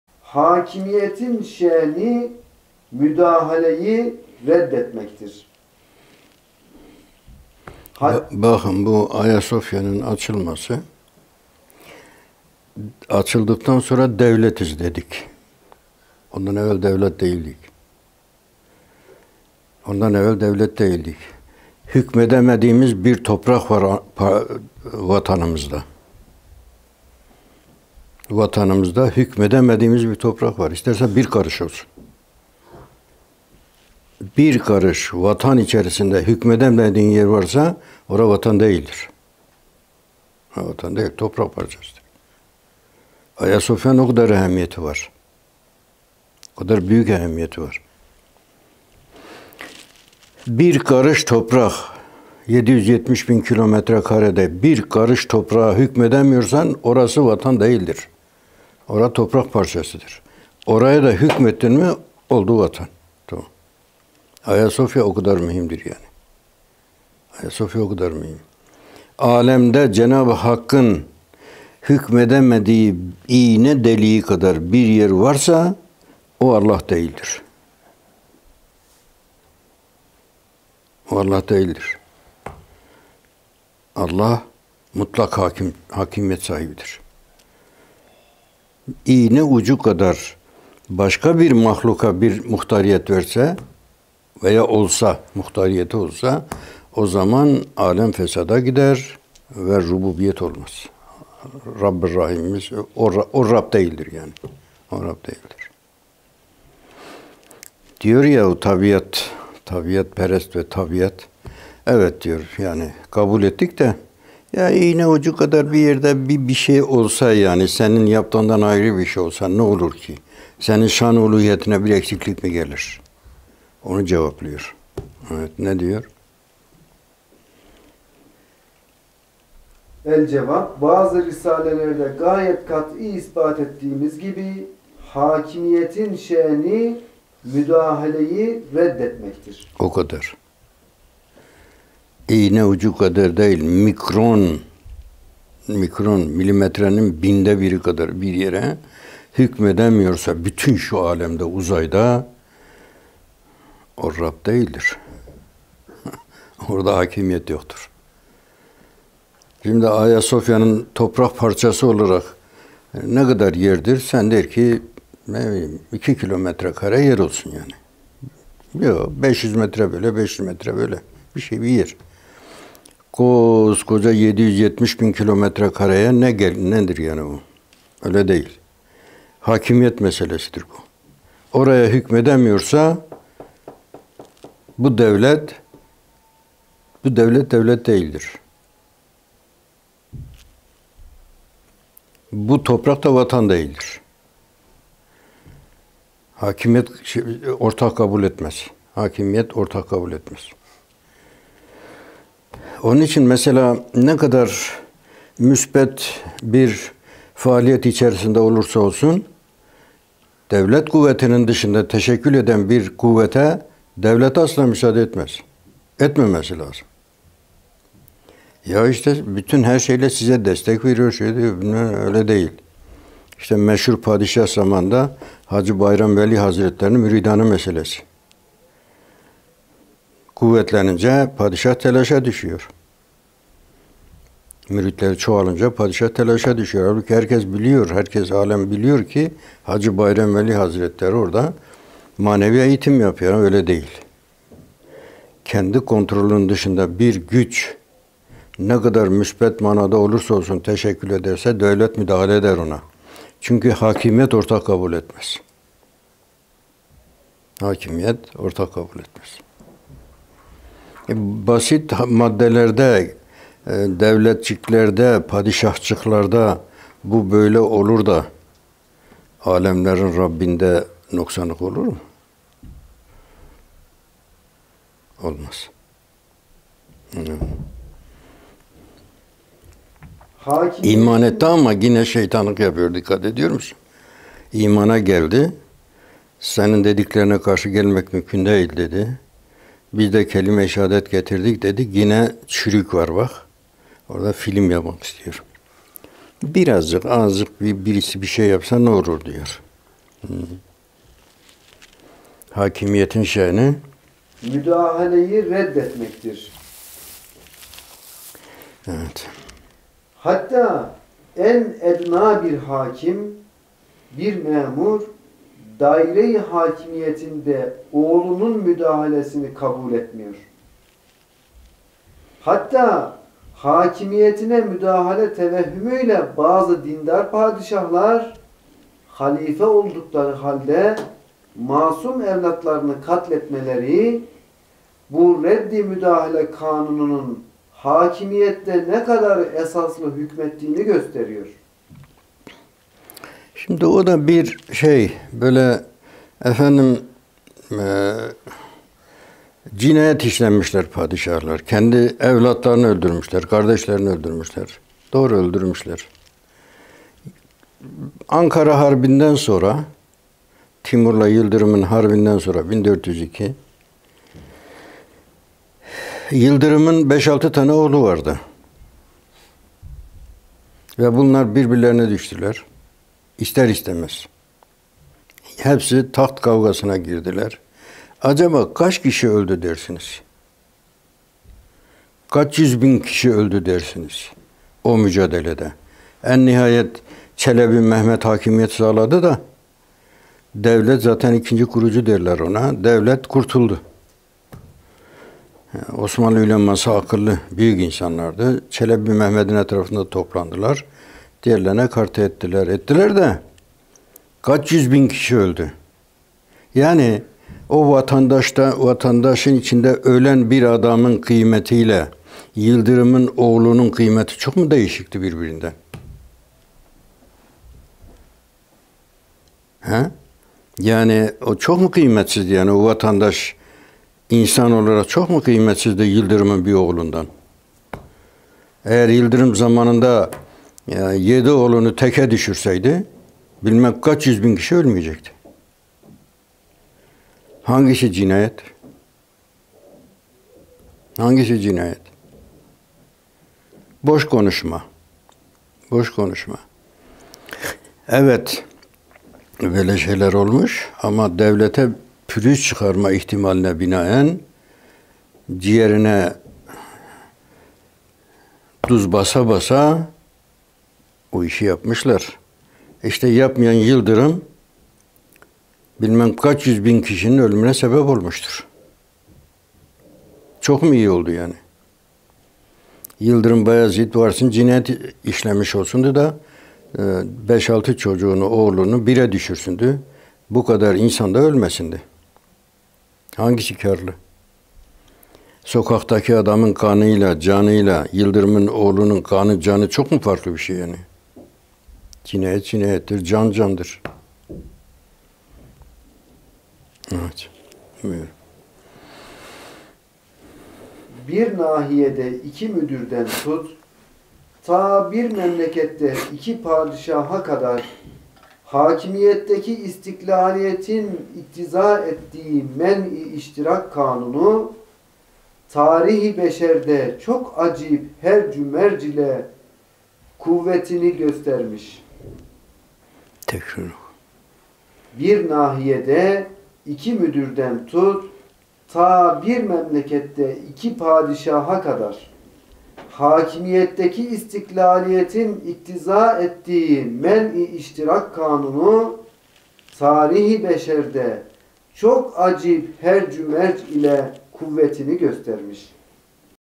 [2401] Hâkimiyetin şe'ni, müdahaleyi reddetmektir. (Müslim Gündüz Efendi)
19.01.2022 Tarihli Aczmendi Risale-i Nur Dersinden
Bazı risalelerde gayet kat'î ispat ettiğimiz gibi, hâkimiyetin şe'ni, müdahaleyi reddetmektir. Hattâ, en ednâ bir hâkim, bir memur, daire-i hâkimiyetinde oğlunun müdahalesini kabul etmiyor. Hattâ, hâkimiyetine müdahale tevehhümüyle, bazı dindar padişahlar, halife oldukları halde mâsum evlâtlarını katletmeleri, bu redd-i müdahale kanununun hâkimiyette ne kadar esaslı hükmettiğini gösteriyor. Bir nahiyede iki müdürden tut, tâ bir memlekette iki padişaha kadar, hâkimiyetteki istiklâliyetin iktiza ettiği men-i iştirak kanunu, tarih-i beşerde çok acip hercümerc ile kuvvetini göstermiş.

Acaba âciz ve muavenete muhtaç insanlardaki âmiriyet ve hâkimiyetin bir gölgesi bu derece müdahaleyi reddetmeyi ve başkasının müdahalesini men etmeyi ve hâkimiyetinde iştirak kabul etmemeyi ve makamında istiklâliyetini nihayet taassupla muhafazaya çalışmayı gör; sonra, hâkimiyet-i mutlaka rububiyet derecesinde; ve âmiriyet-i mutlaka ulûhiyet derecesinde; ve istiklâliyet-i mutlaka ehadiyet derecesinde; ve istiğnâ-yı mutlak kadîriyet-i mutlaka derecesinde bir Zât-ı Zülcelâlde, bu redd-i müdahale ve men-i iştirak ve tard-ı şerik, ne derece o hâkimiyetin zarurî bir lâzımı ve vâcip bir muktezası olduğunu, kıyas edebilirsen et.